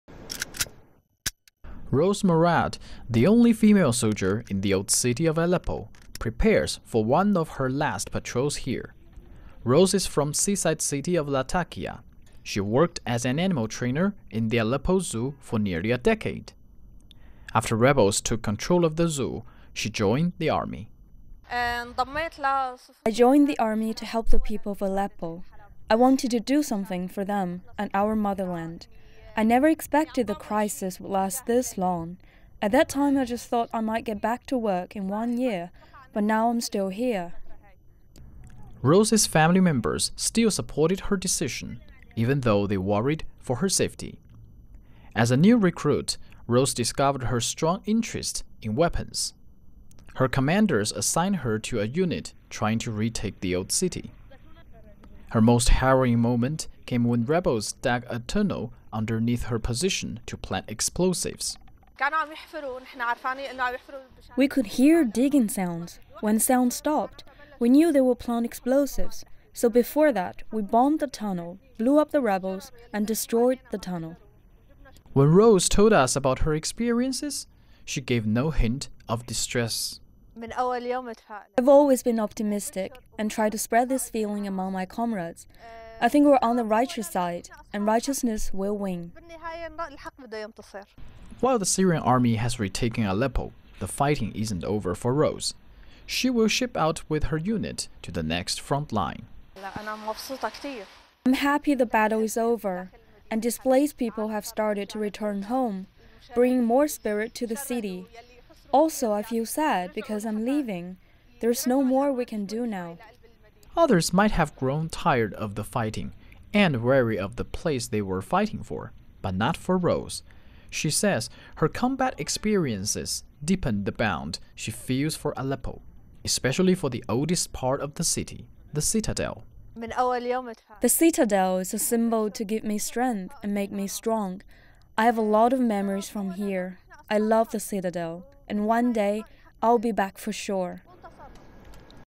A ‘rose’ fights to retake Aleppo
Hundreds of thousands of people, both civilians and soldiers, have died in the battle for Syria. One of those fighting on the frontline in Aleppo was a soldier who says she never lost hope. CGTN’s Xu Dezhi brings us the extraordinary story of a woman who has fought hard for her city and her country.